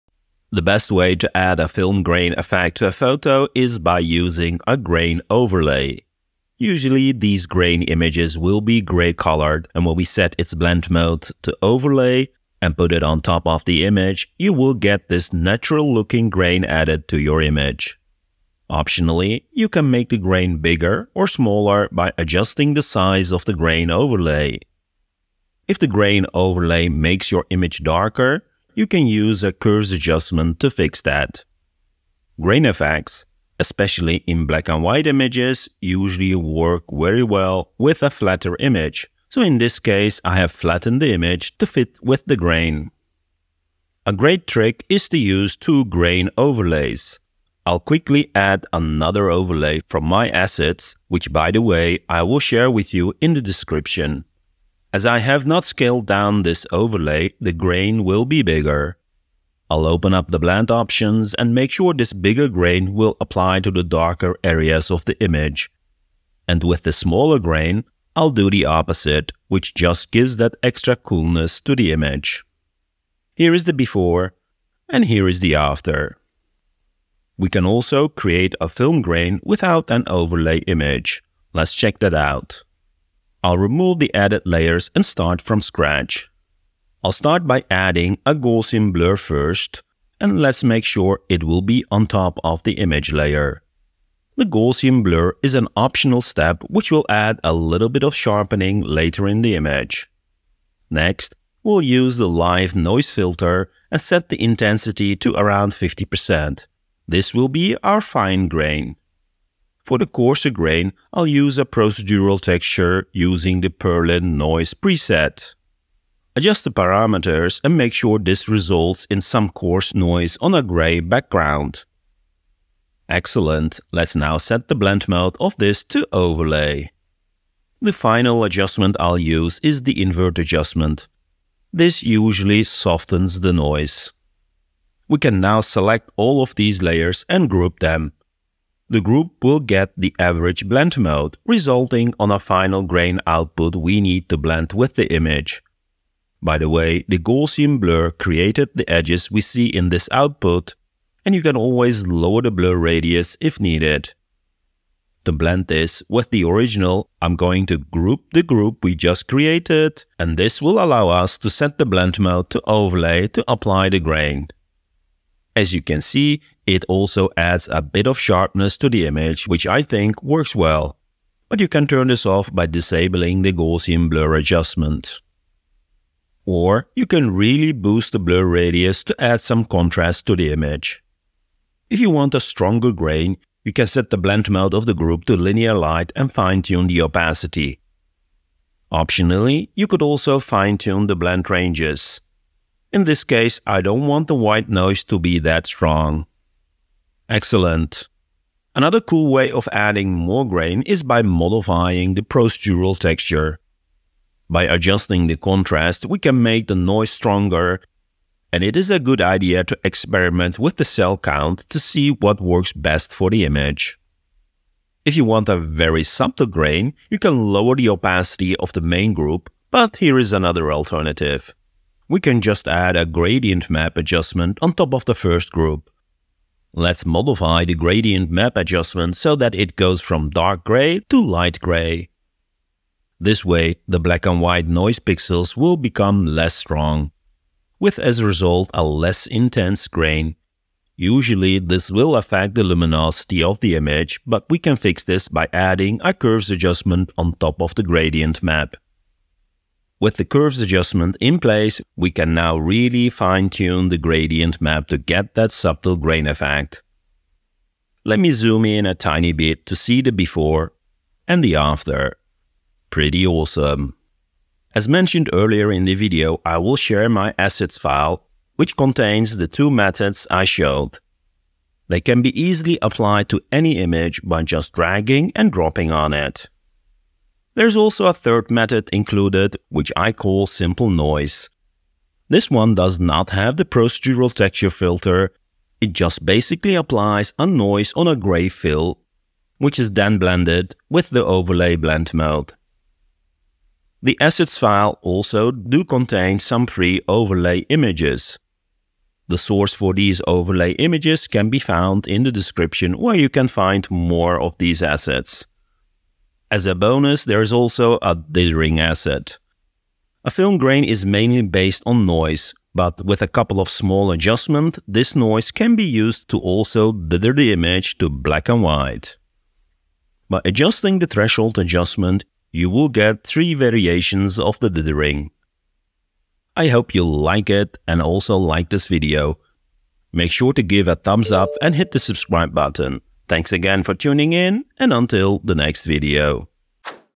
Add HIGH quality FILM Grain to photos using Affinity Photo
In this video we dig into applying film grains to photos. Usually it is an artistic effect that adds a bit of an analog feeling to your photo.